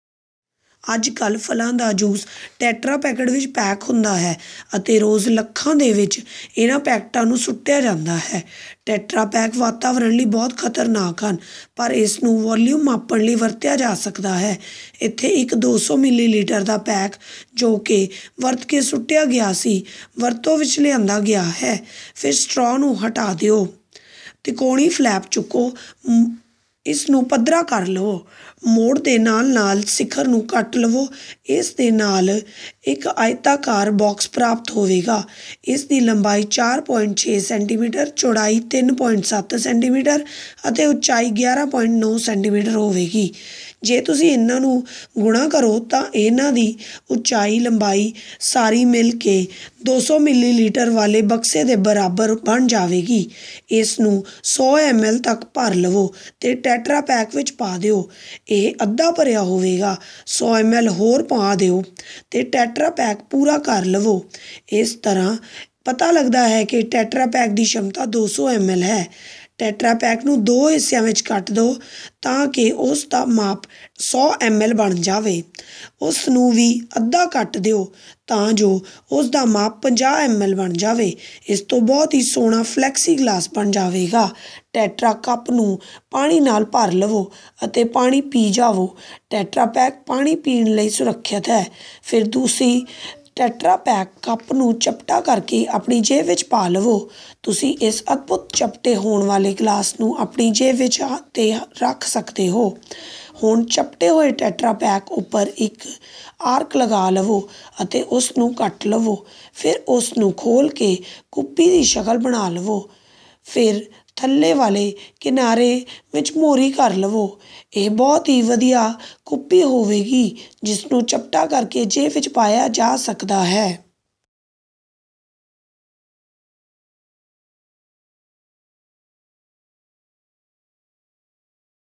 TETRA VOLUME | Punjabi
Fruit juice is nowadays packed in Tetrapaks and millions of these packets are trashed every day. Tetrapaks are mighty bad for the environment but they can be used for measuring VOLUME.
Here is a 200-ml packet which has been used and thrown. Remove the straw, lift the triangular flaps and flatten the top. Then cut the top along the fold. You will get a rectangular box with a capacity to hold 200-ml of water. The length is 4.6-cm, width is 3.7-cm and the height is 11.9-cm. If you multiply them you will get very close to 200-ml – the volume of the box.
Fill a regular volume cylinder and fill it to 100-ml. Pour in the tetrapak and it will be half full. Pour another 100-ml and then the tetrapak will be filled to the top. This proves that the tetrapak had a capacity of 200-ml.
Cut a tetrapak in half along the height to make a 100-ml measure. Cut another one in quarter to make a 50-ml measure.
You can make a lovely FLEXI-GLASS. Fill your tetra-cup with water and drink it. Tetrapak cup is safe for drinking. Later you can flatten the tetrapak cup and stow it away in your pocket. You can carry this amazing COLLAPSIBLE GLASS in your pocket.
Now mark an arc on a flattened tetrapak and cut along the curve. Then open and shape it into a funnel. Then nip the bottom corner to make a hole. This is a wonderful improvised FUNNEL which again can be flattened and tucked away in your pocket.